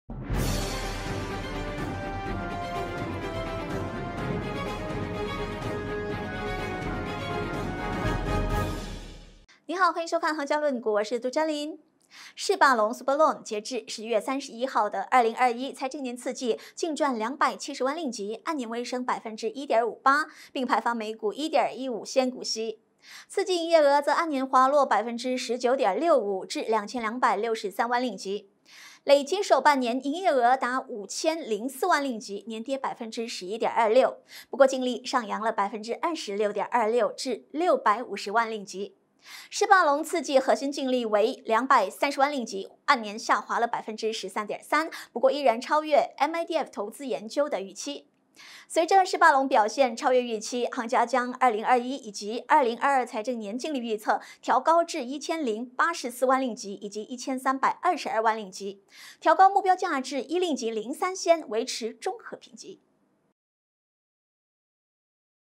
行家论股】世霸龙 次季财报胜预期
世霸龙（SUPERLN，7235，主板工业股）截至10月31日的2021财年次季，净赚270万4000令吉，按年微升1.58%，并派发每股1.15仙股息。